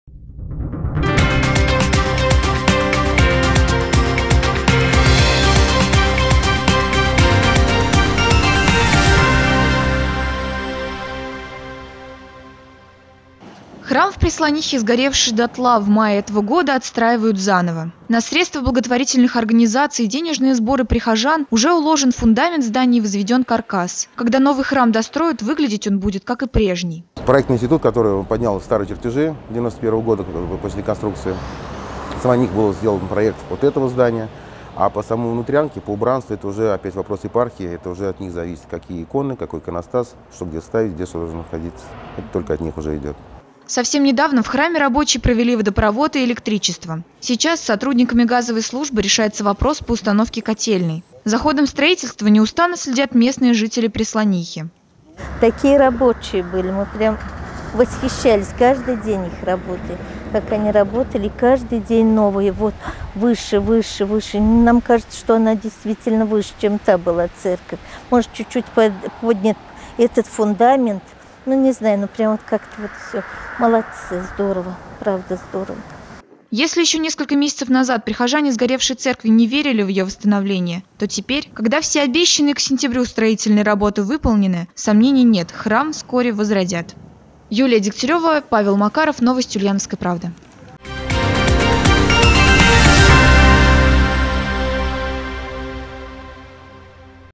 Возрождение церкви
Храм в Прислонихе сгоревший дотла в мае этого года отстраивают заново. Уже уложен фундамент здания, возведен каркас, а совсем недавно в рабочие провели водопровод и электричество. Сейчас с сотрудниками газовой службы решается вопрос по установке котельной. По факту все обещанные к сентябрю строительные работы выполнены. За ходом строительства неустанно следят местные жители Прислонихи, которые уверены, что их любимый храм, сгоревший этой весной как свеча, возродится из пепла в скором времени.